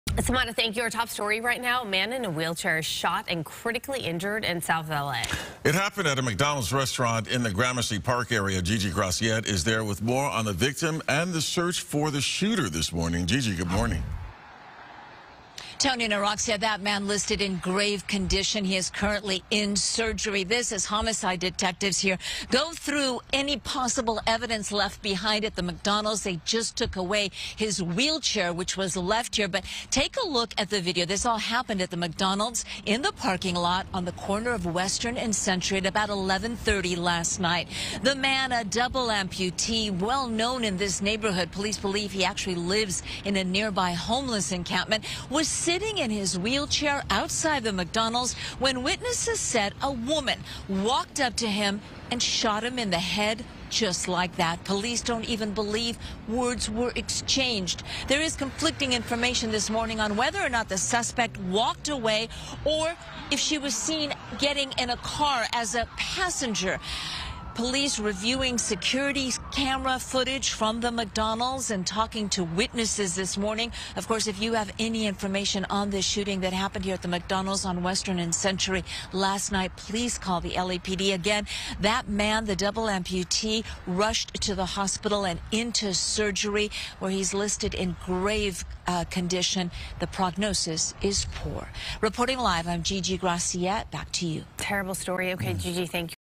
Man in a wheelchair shot in South LA
The victim is in grave condition.